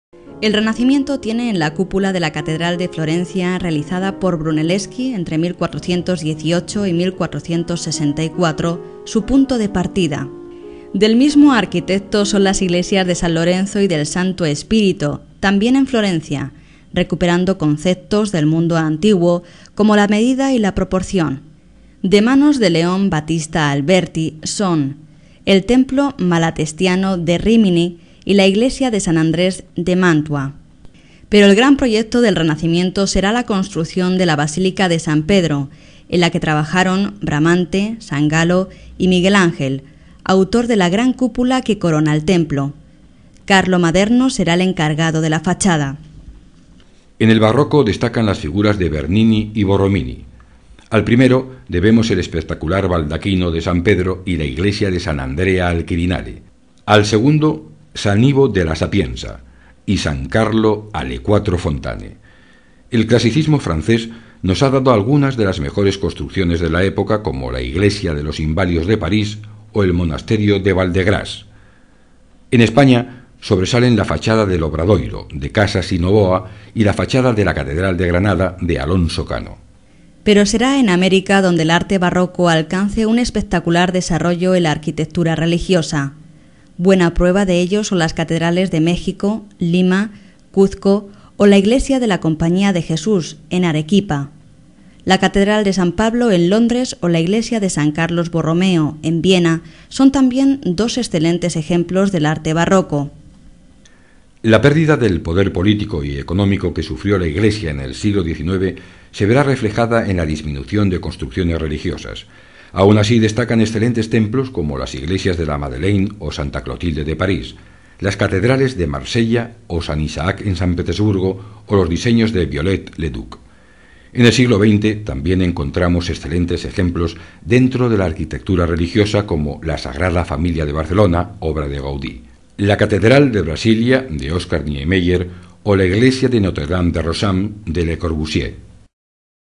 Iglesias modernas y contemporáneas
El Renacimiento tiene en la cúpula de la catedral de Florencia, realizada por Brunelleschi entre 1418 y 1464, su punto de partida. Del mismo arquitecto son las iglesias de San Lorenzo y del Santo Spirito, también en Florencia, recuperando conceptos del mundo antiguo como la medida y la proporción. De manos de Leon Battista Alberti son el Templo Malatestiano de Rímini y la iglesia de San Andrés de Mantua. Pero el gran proyecto del Renacimiento será la construcción de la basílica de San Pedro, en la que trabajaron Bramante, Sangallo y Miguel Angel, autor de la gran cúpula que corona el templo. Carlo Maderno será el encargado de la fachada.